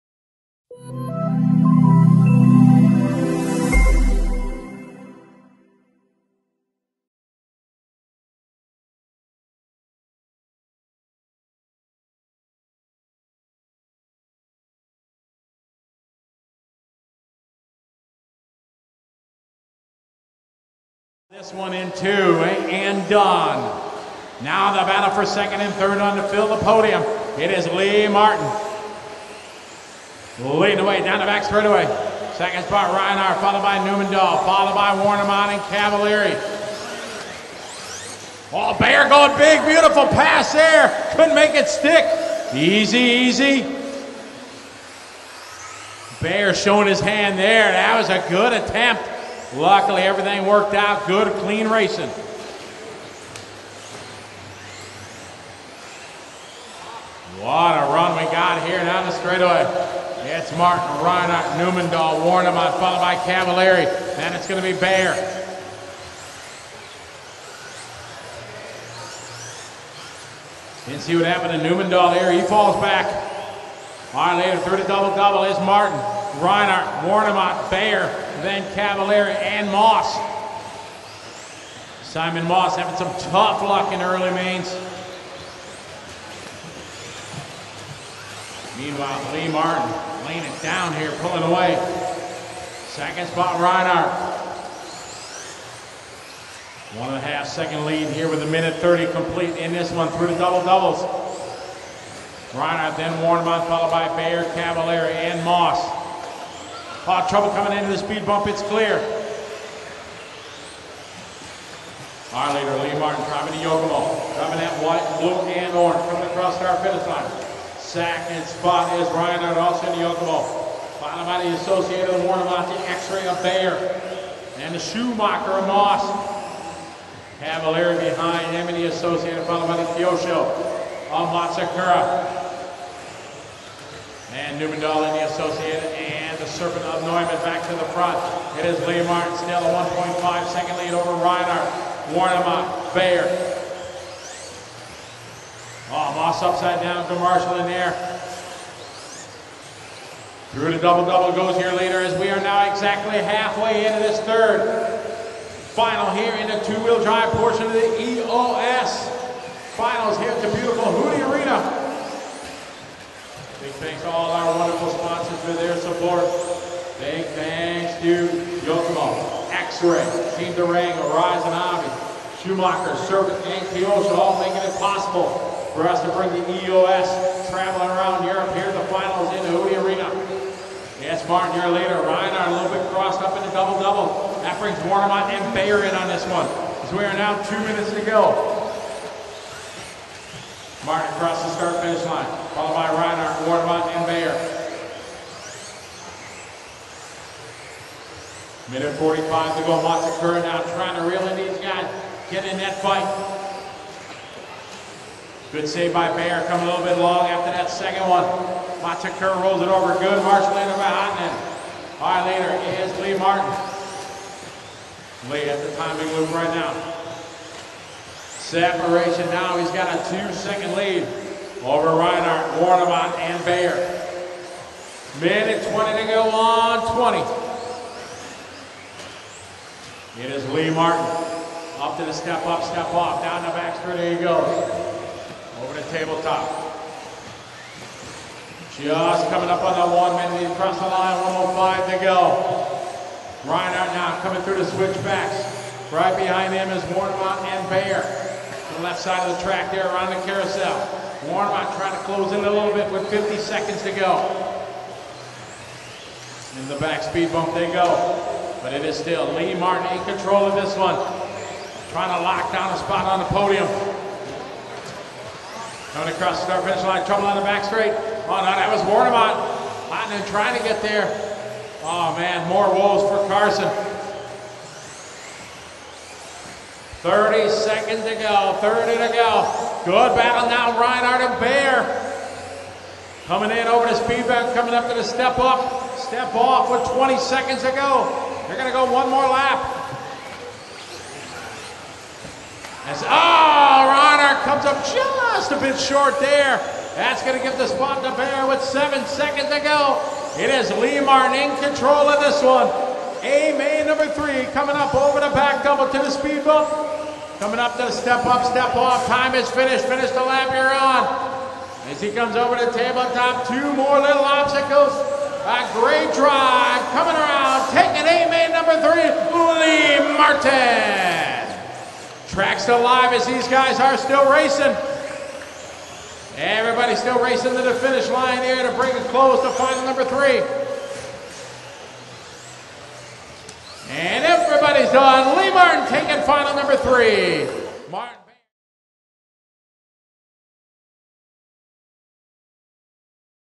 2014/15 Euro Offroad Series Rd4 - 2wd A-Main Leg 3
Action from 2wd A-Main Leg 3 at the 4th round of the 2014/15 Euro Offroad Series in Trencin, Slovakia.